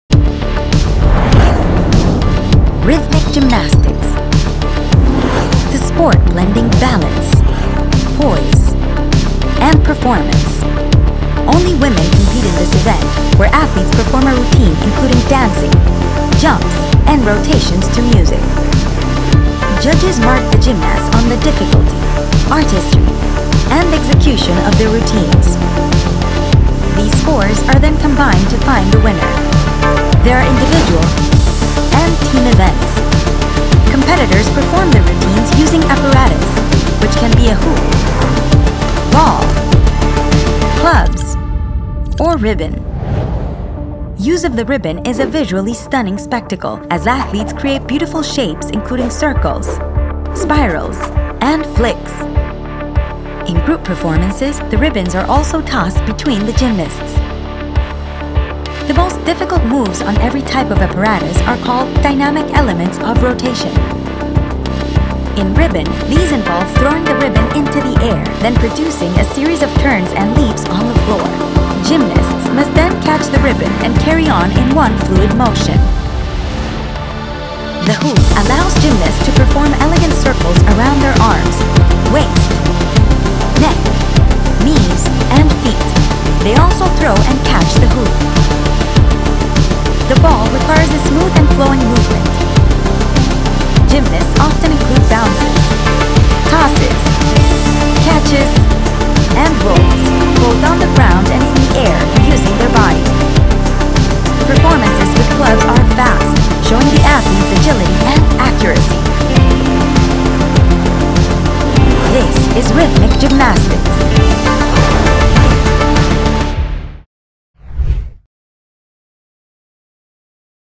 Know all about Rhythmic Gymnastics - An Olympic Sport Guide | Paris 2024 | JioCinema & Sports18
A sport that is only performed by Women and this sport blends with balance, poise, and performance 🤸‍♂️

Know everything about Fencing and get ready for ParisOlympics2024 👉 Catch it live from July 26th on Sports18 1, 1 HD, and JioCinema 🙌